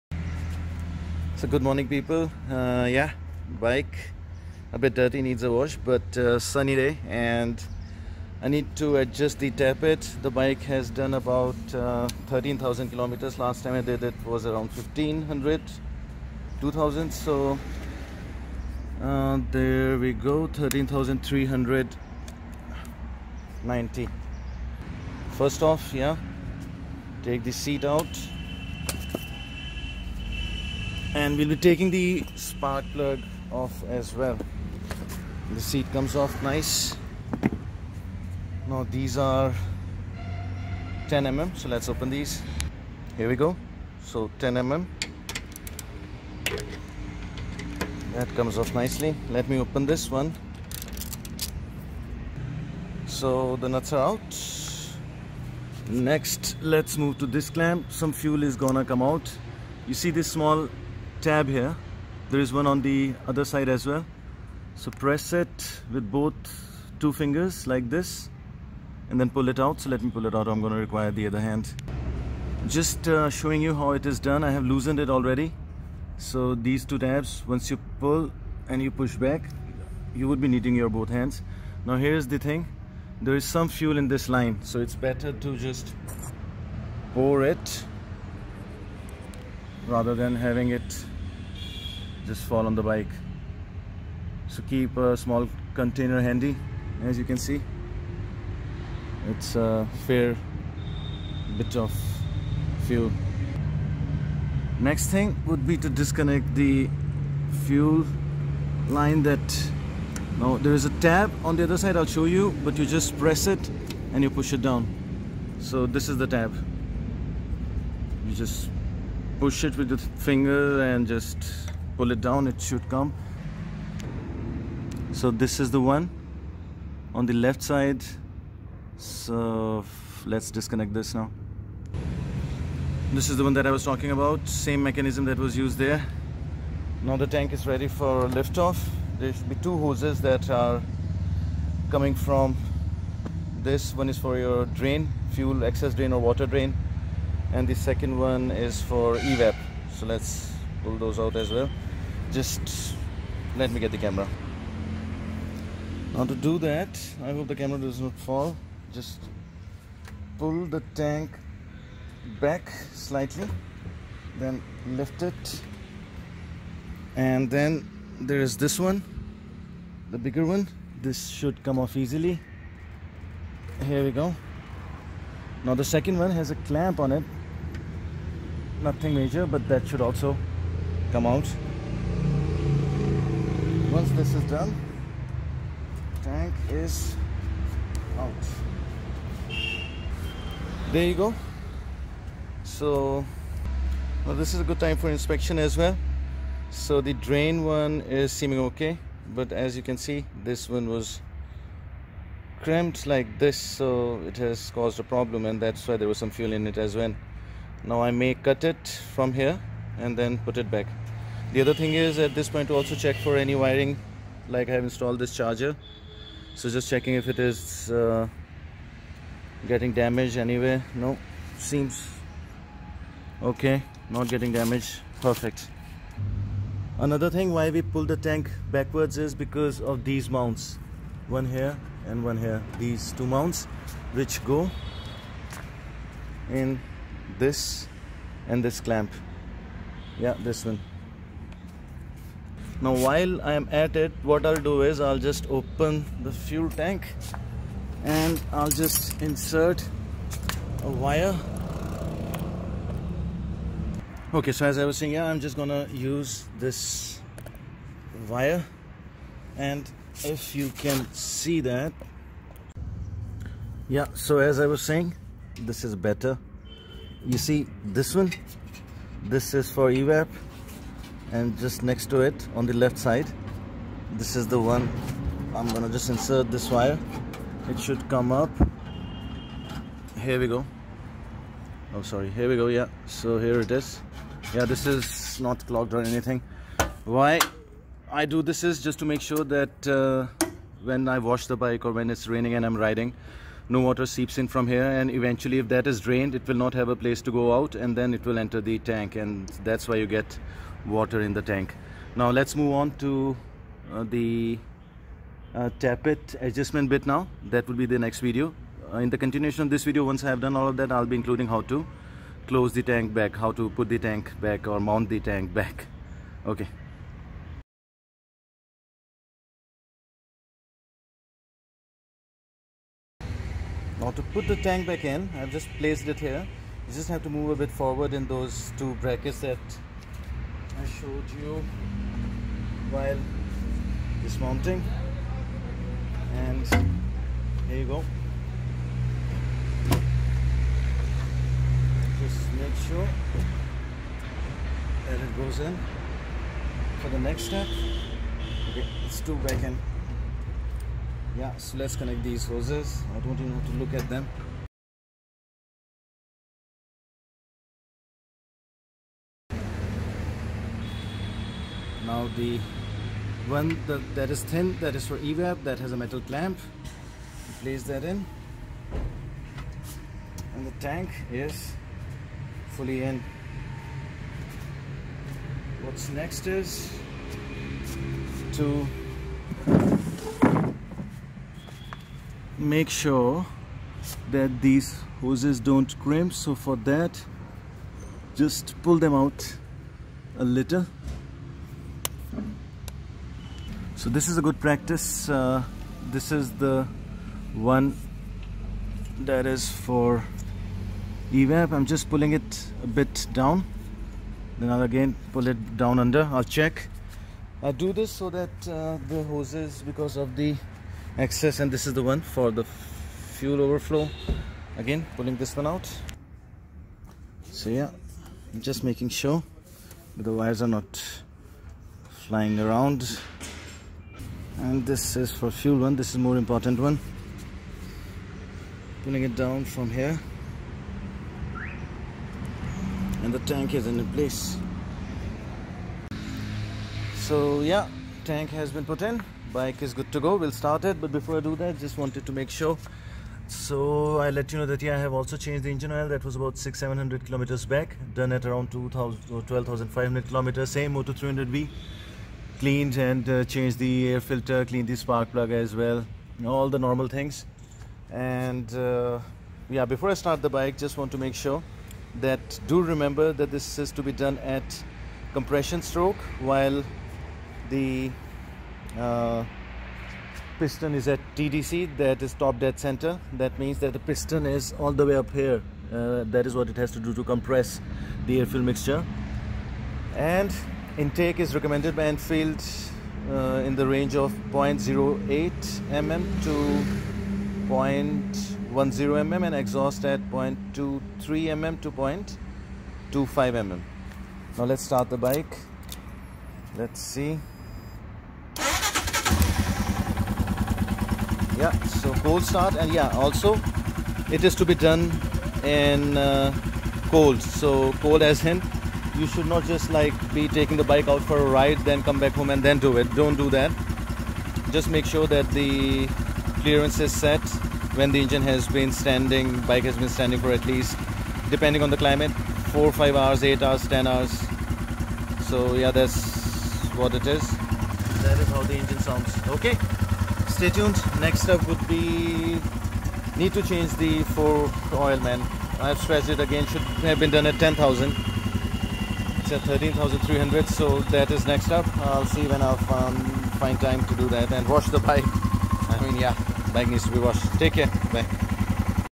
Royal Enfield Himalayan Tank Dismount | Remount | Plus Water in the Tank | How to Avoid | DIY
Hey
This section covers removal or dismounting of tank on the Himalayan along with remounting.
I have also included some precautionary steps to make sure we dont get water in the tank.
Cheers :)